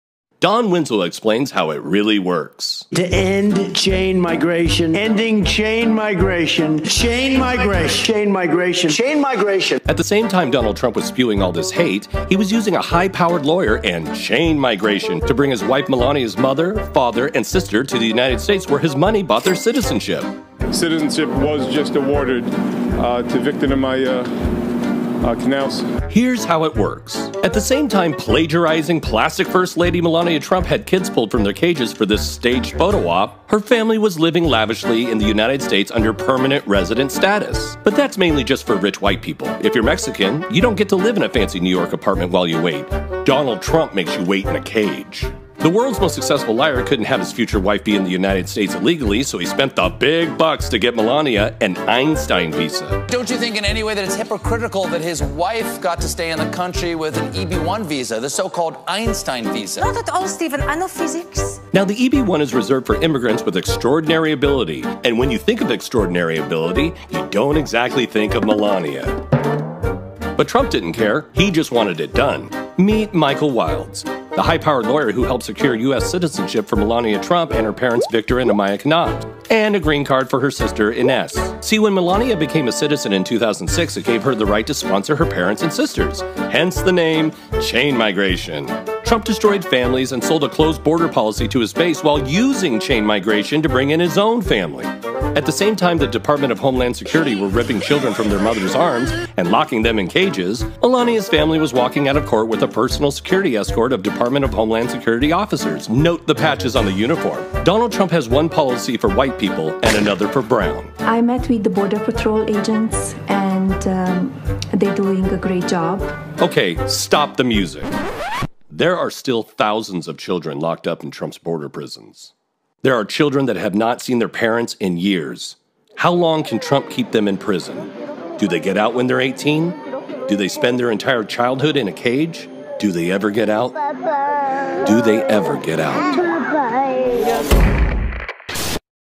Chain Migration How It Works If You Have Money
Chain migration explained if you have money or some inside connections.